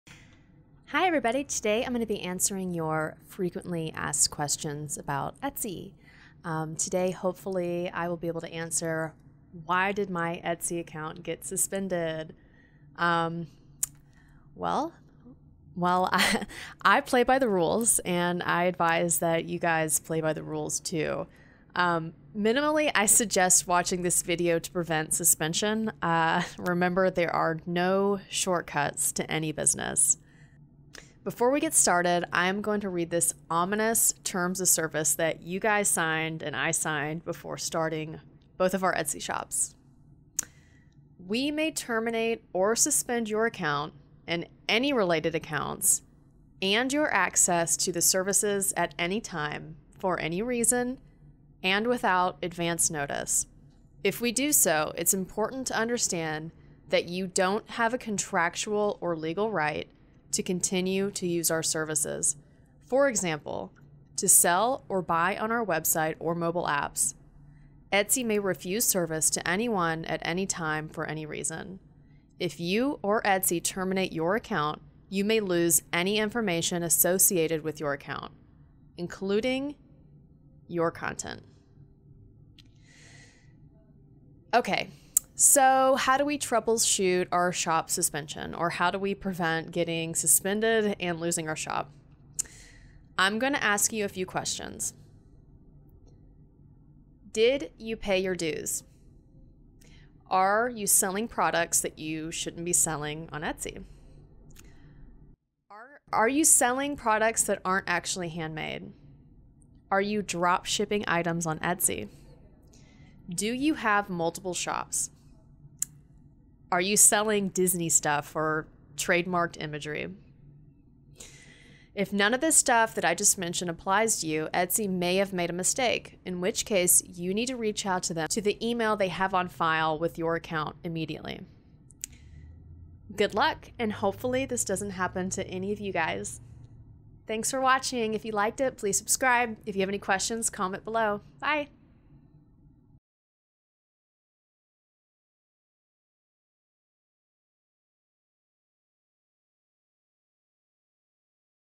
Why Your Etsy Shop Got Suspended
Hi! My name is Bailey. I am the founder of Ringcrush, and of the "Million Dollar Etsy Shop". Today I am going over frequently asked questions about Etsy. I am briefly covering common reasons Etsy sellers lose their Etsy shop. If you need further details, feel free to comment below, I am happy to elaborate as much as I can, but for the sake of brevity, I generally try to keep videos to the point.

I'll teach you how to build your own Million Dollar Etsy Shop!